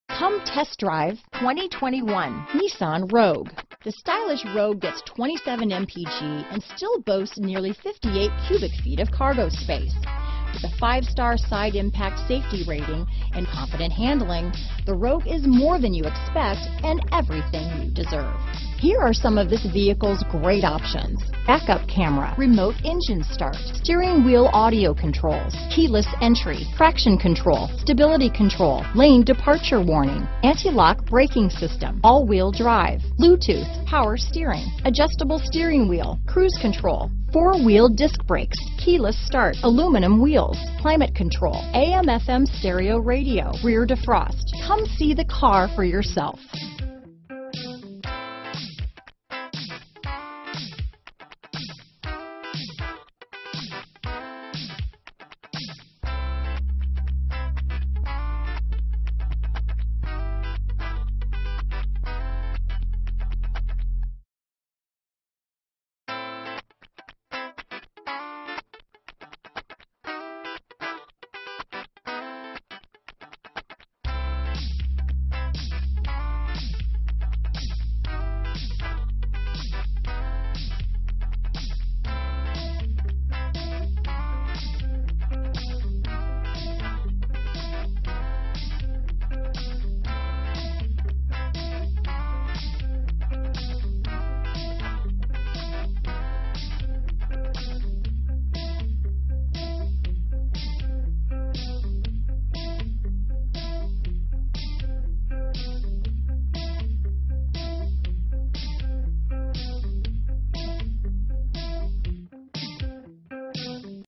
2021 Nissan Rogue Albany, Colonie, Latham, Johnstown, Glens Falls, NY N21R0007
Pearl White Tricoat New 2021 Nissan Rogue available in Albany, New York at Destination Nissan. Servicing the Colonie, Latham, Johnstown, Glens Falls, NY area.

Destination Nissan
770 Central Ave